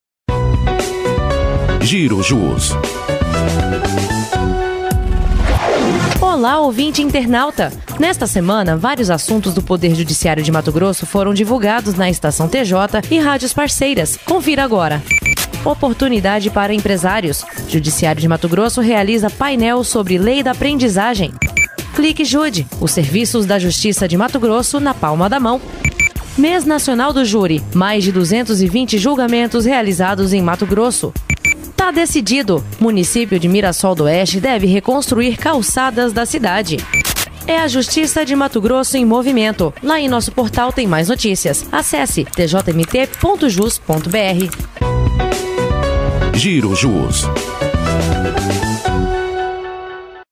Estação TJ: Giro de Notícias
Olá, ouvinte internauta!

Nesta semana, vários assuntos do Poder Judiciário de Mato Grosso foram divulgados na Estação TJ e rádios parceiras. Confira agora!